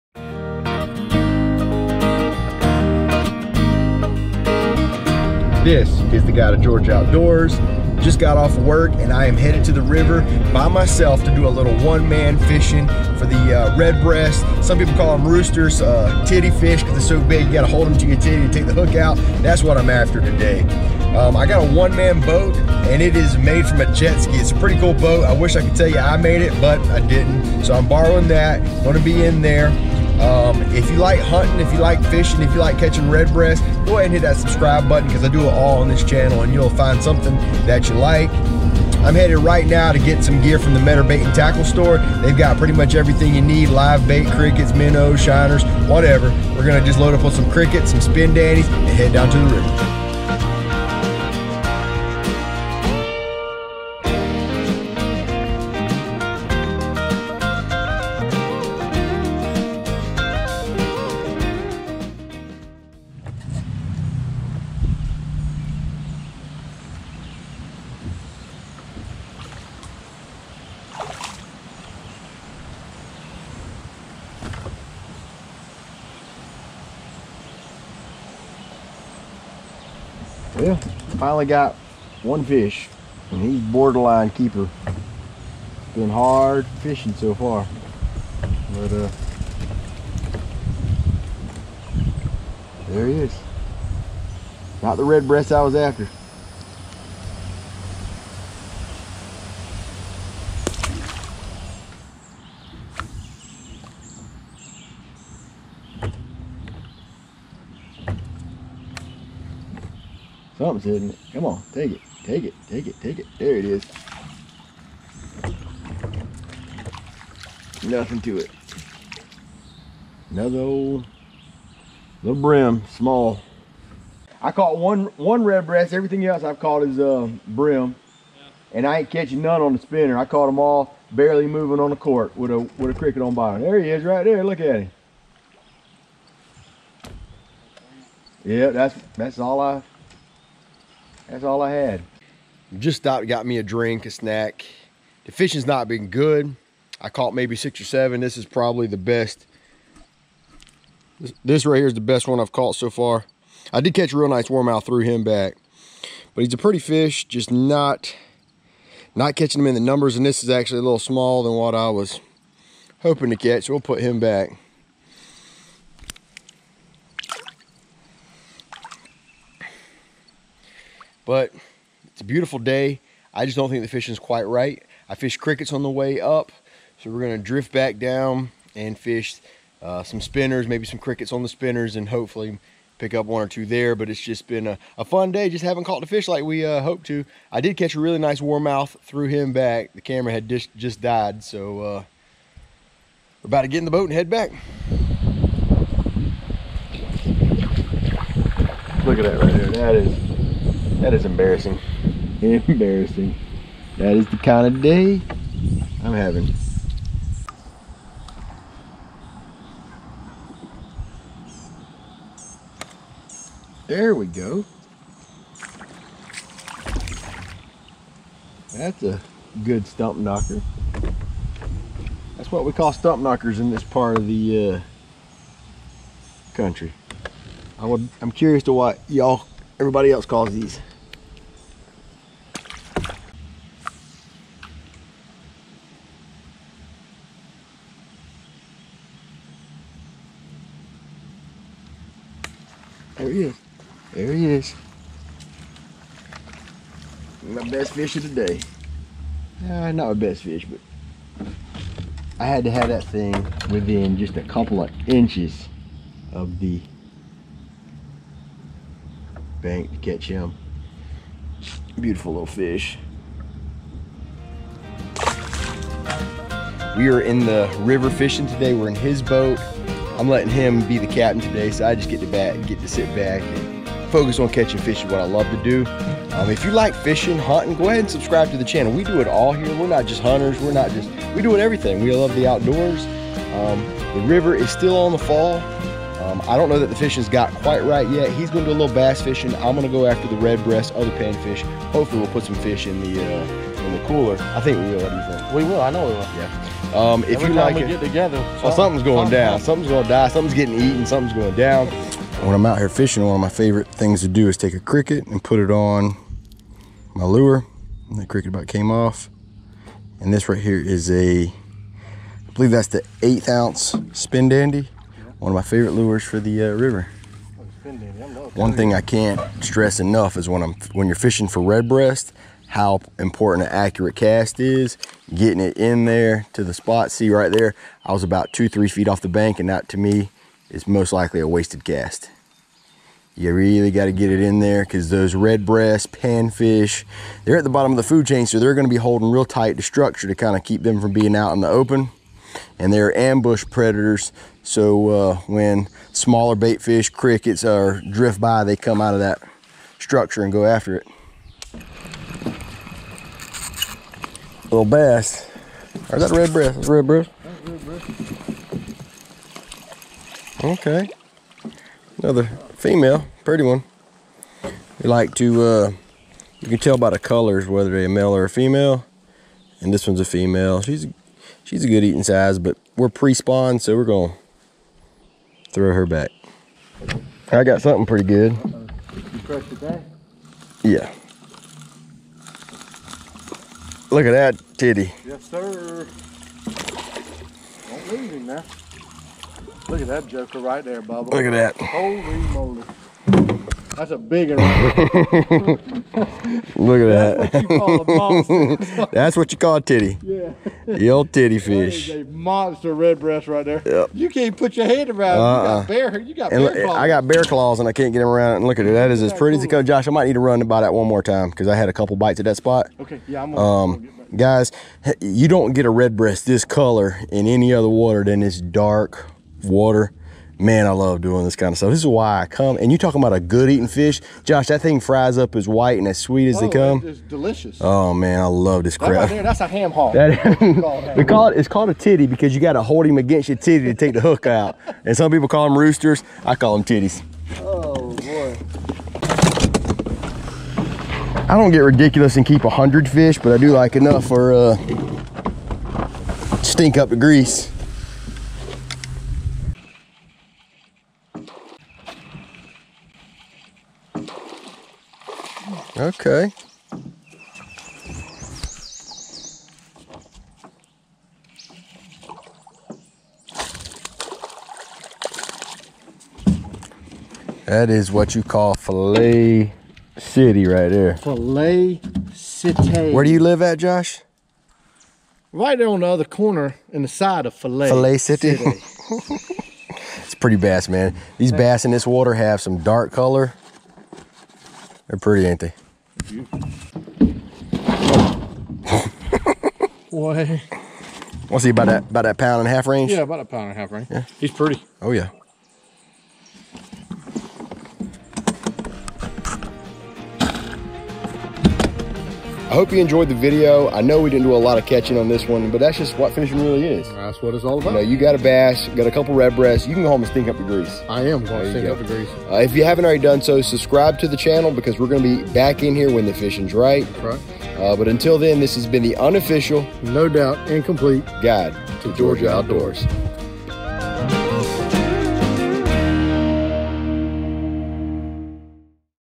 Catching Big Redbreast! (Rooster/Titty) *SE Georgia
The G2GO crew hit the dark, slow moving water of the Canoochee river to go after some big Redbreast. The fish are the brightest color in the darkest water and although we didn't tear them up we did catch a few. Can't wait to get back. Thanks for checking out the channel, we really love making these videos.
Thanks
-G2GO Crew